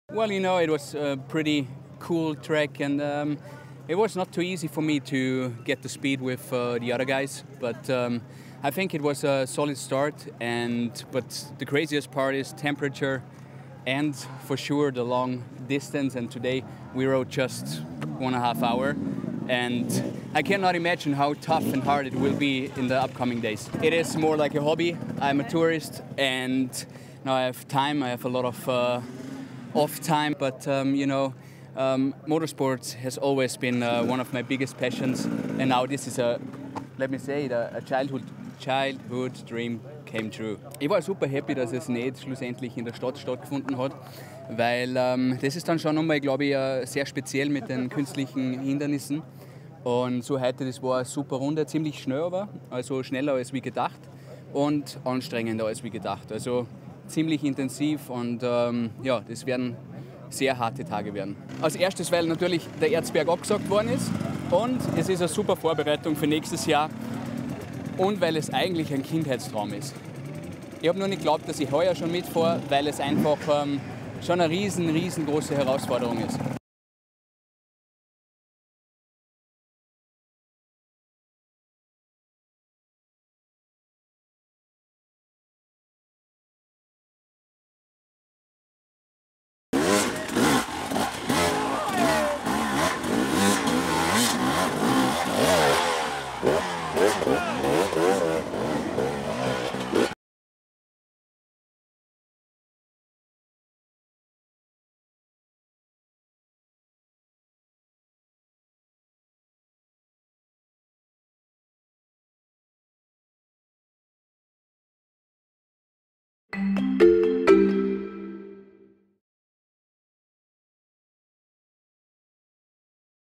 Romaniacs Hirscher
Der achtfache Skiweltcupsieger Marcel Hirscher will sich nun auch auf zwei Rädern beweisen. Er startet beim Enduro-Rennen Red Bull Romaniacs. Mehr wissen